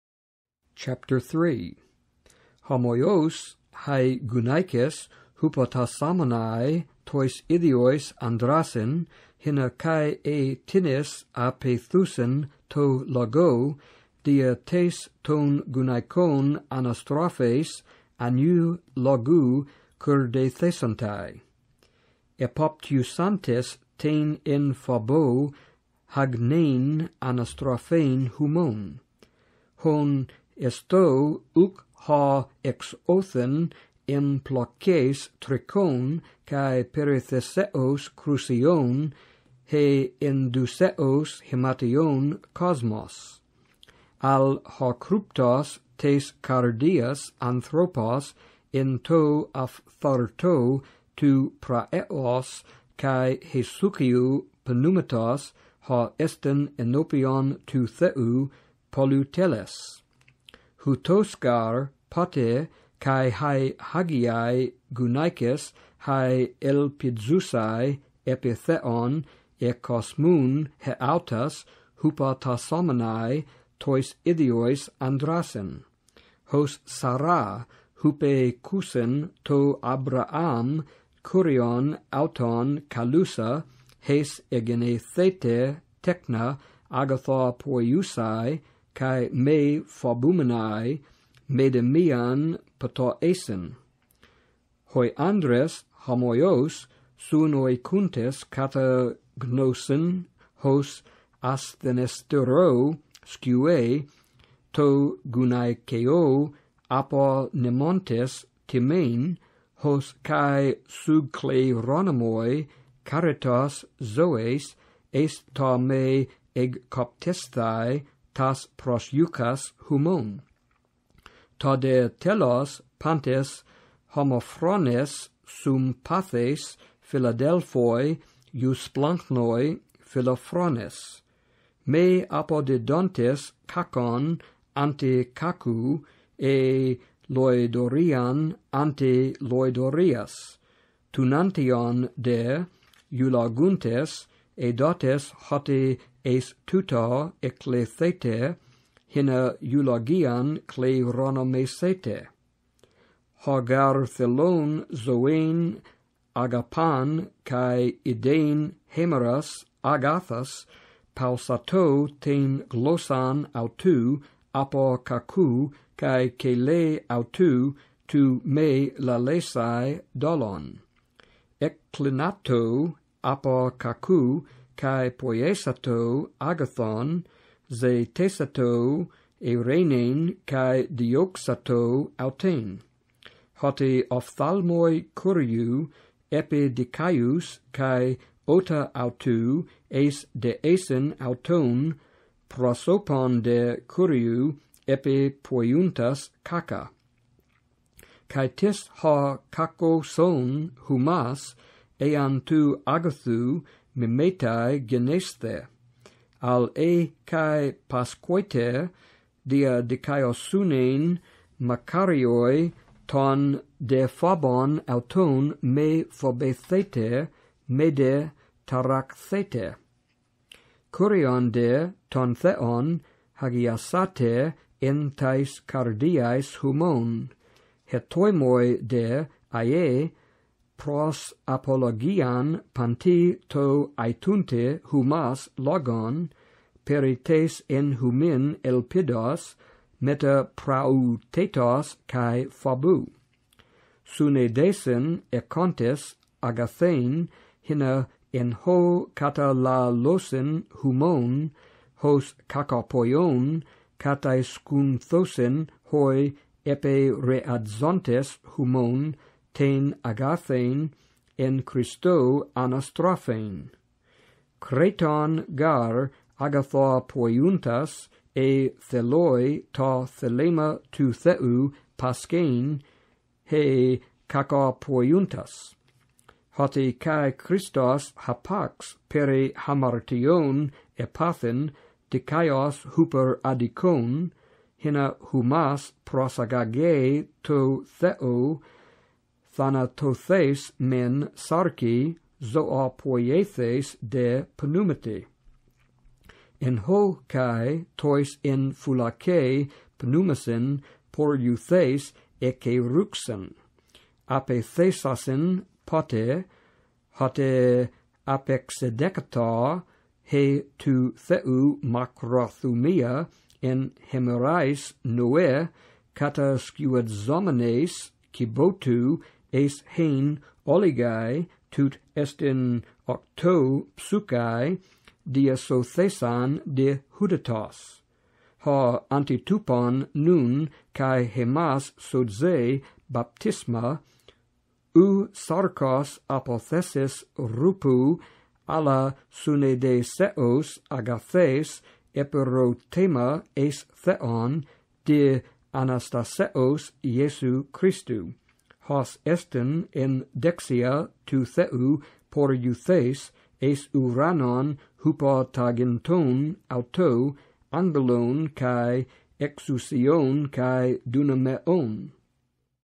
I Peter 3 in Greek (Maurice Robinson)
Maurice Robinson reeds I Peter 3 in Greek (Robinson-Pierpont 2005 edition).